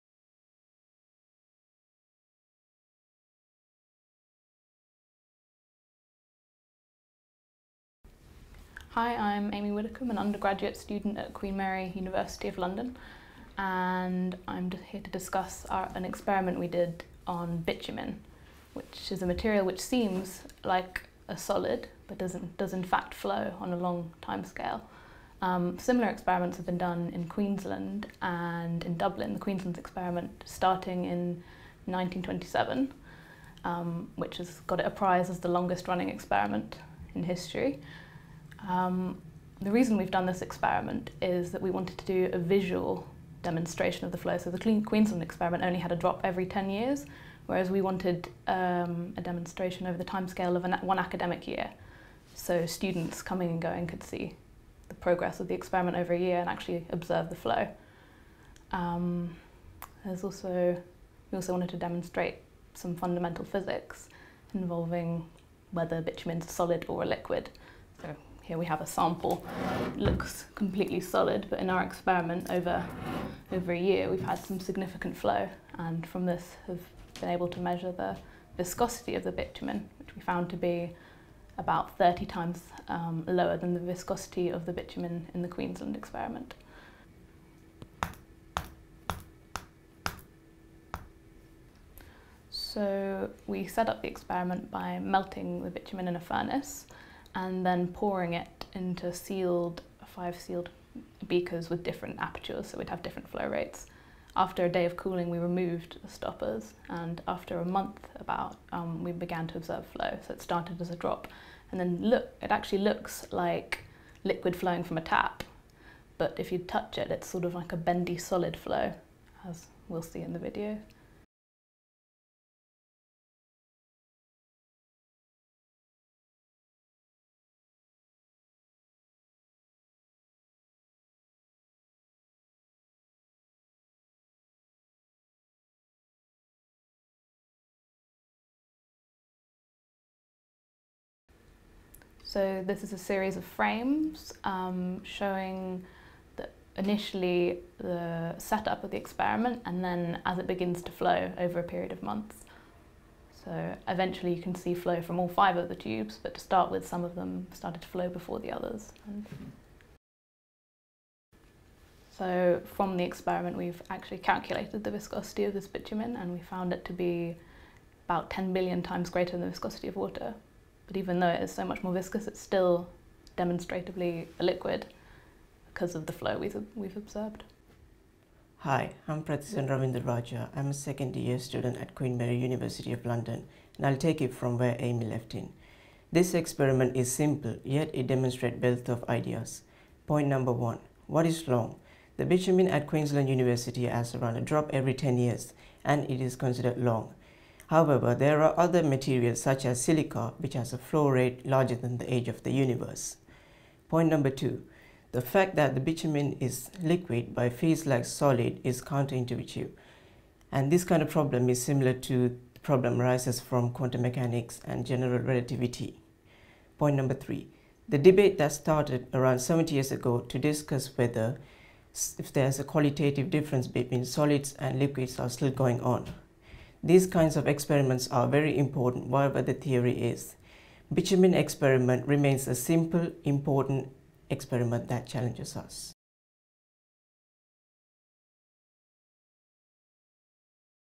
Nature of solids and liquids explored through new pitch drop experiment
This video abstract explains the latest research from physicists at Queen Mary University of London, who have have set up a new pitch drop experiment for students to explore the difference between solid and liquids.